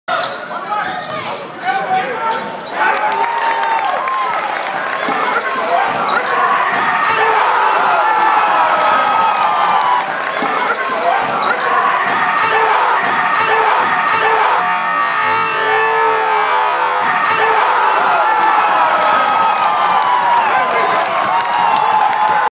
Colonial Forge's Winston Burgess' block & 1-hand cock back backboard shaking HAMMER!
Colonial Forge High School's Winston Burgess blocks a shot, runs the floor and cocks back a powerful 1-hand dunk that rocks the backboard against C.D. Hylton High School.